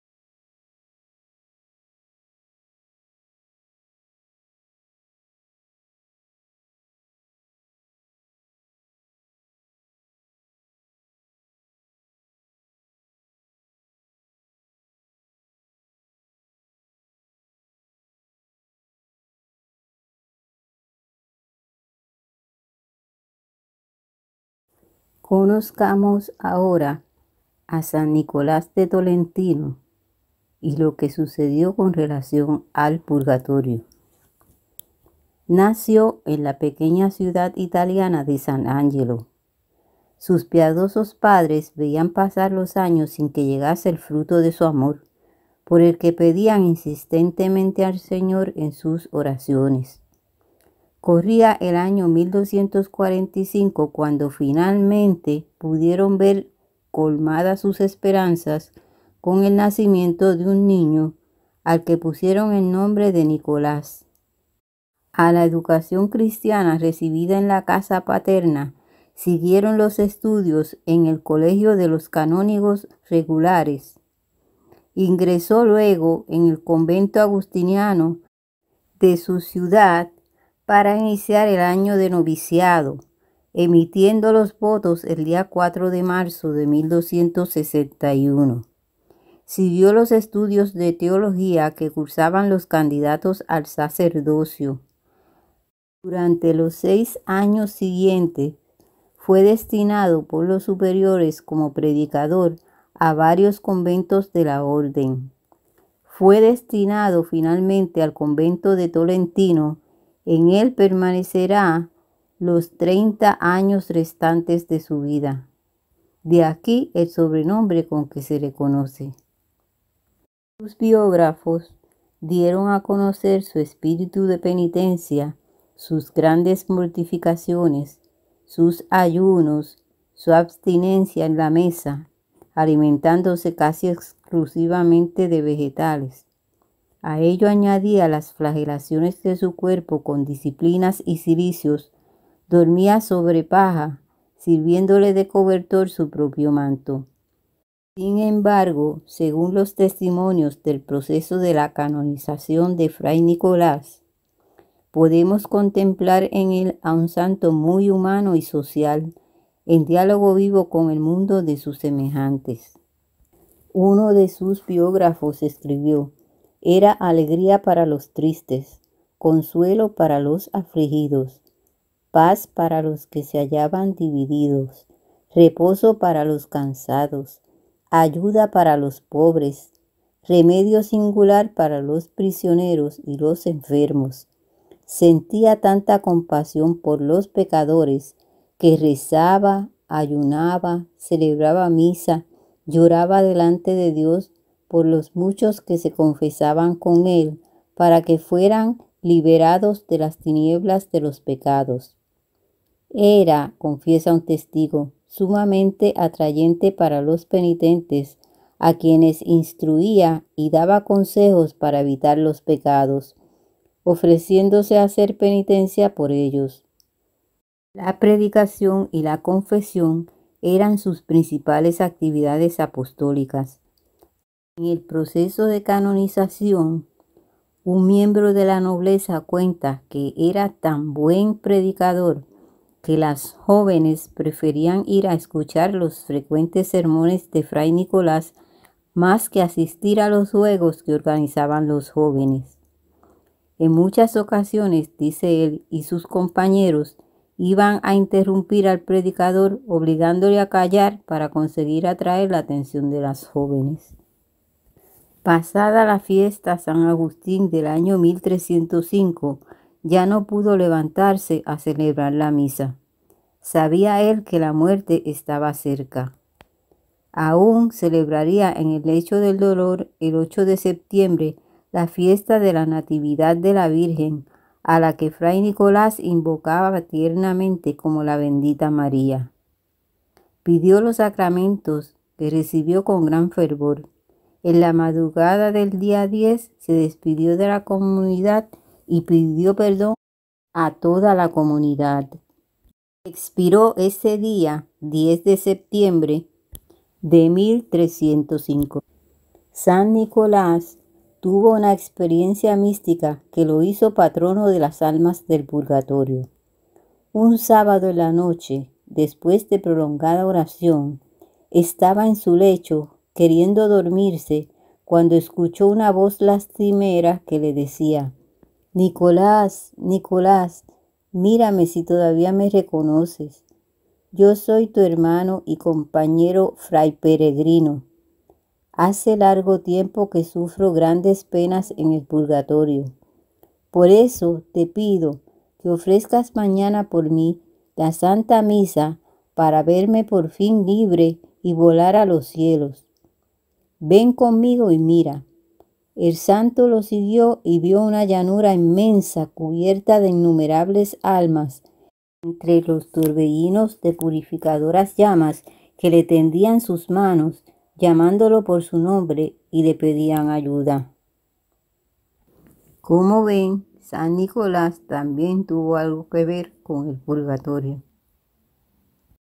SAN NICOLÁS DE TOLENTINO ---Y EL PURGATORIO
ANTESALA A LA VIGILIA DE LOS DÍAS 3 Y 4 DE JULIO ,2020.
ESTA VIGILIA OFRECERÁ SUS ORACIONES PRINCIPALMENTE POR
LAS ALMAS DEL PURGATORIO.